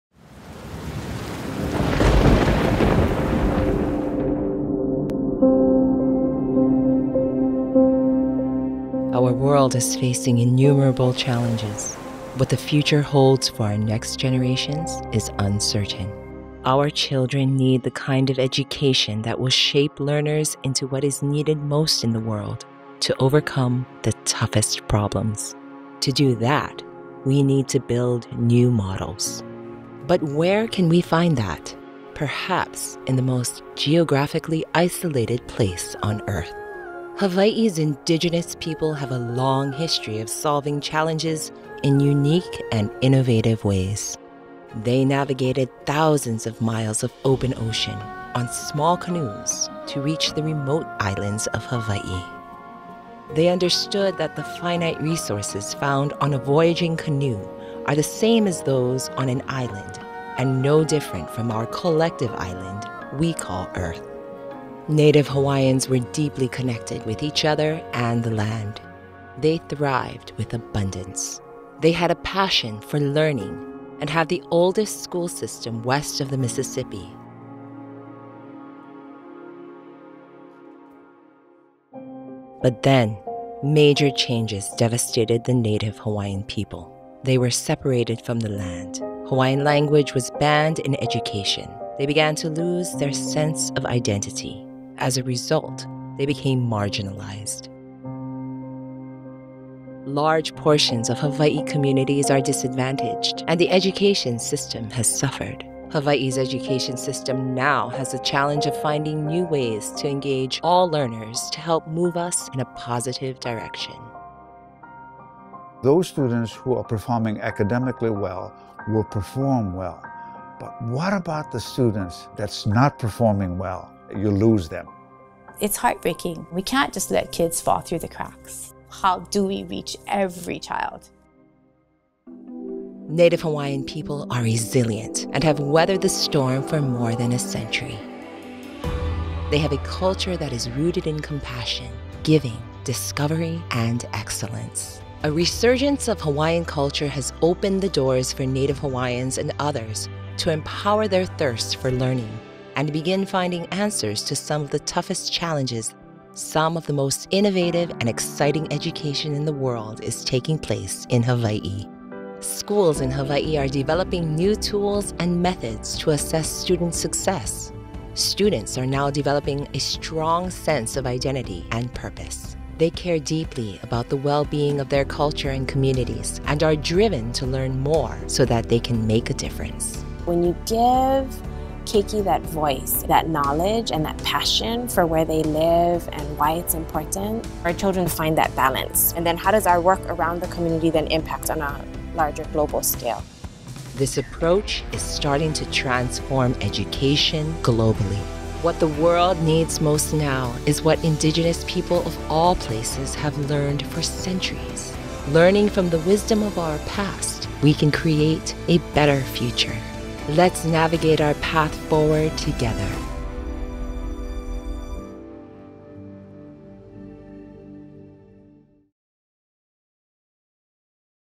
Hawaii Education for the World
When the native Hawaiian community was forcefully disconnected from their culture, they became more marginalized. But with the current resurgence of the Hawaiian culture today, it has opened doors to more possibilities and opportunities for the Hawaiian people by innovating through the land and compassion with others. By learning from the wisdom of our past, we can create a better future.

We worked with Kamehameha Schools to produce this video to inspire support for Native Hawaiian education development. Hawaiʻi's indigenous people have a long history of solving challenges in unique and innovative ways that can inform and educate the next generations globally to face some of the toughest challenges.

We had support from Kanaeokana, Hokulea, Malama Honua PCS, Aloha School on Kauaʻi, and ʻŌiwi TV. Voice Narration by Starr Kalahiki.

SOCIAL